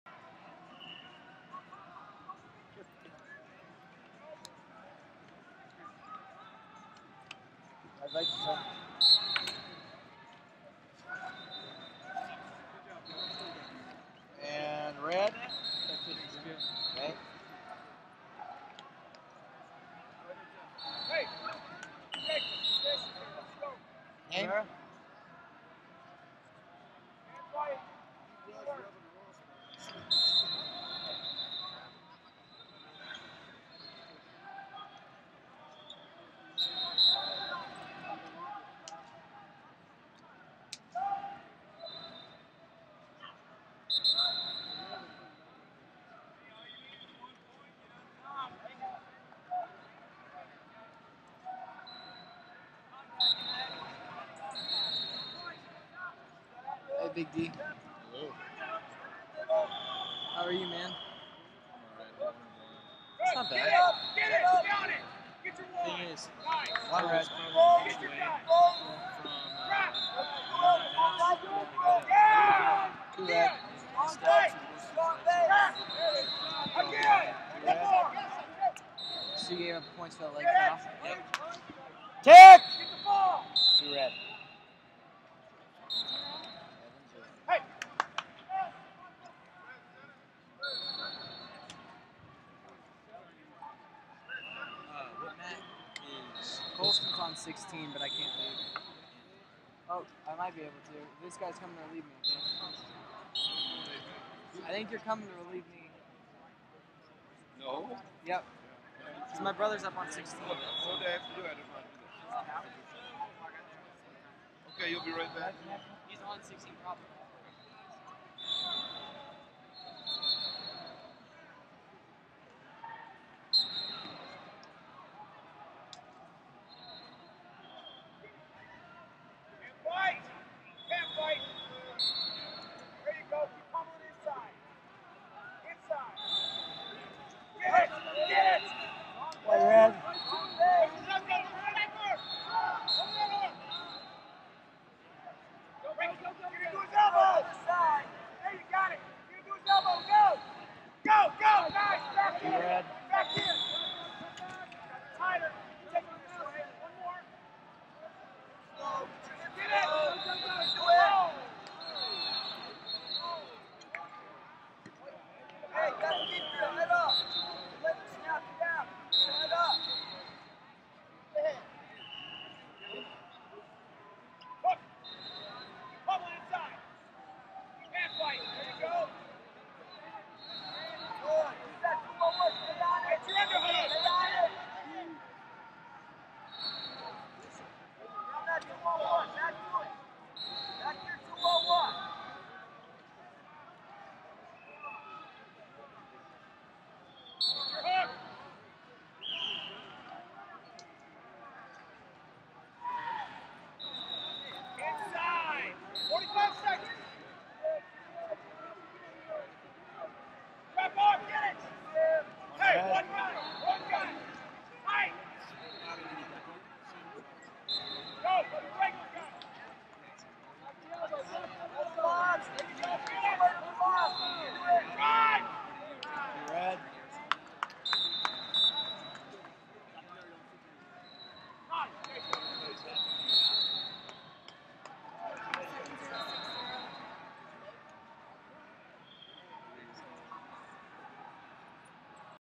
Fargo 2012 126 Round 4: Jacob Rubio (Texas) vs. Rafael Hererra (California)
Round 4 match of the 2012 USAW Cadet Greco Nationals with Jacob Rubio from Texas vs Rafael Hererra from California at 126 lbs.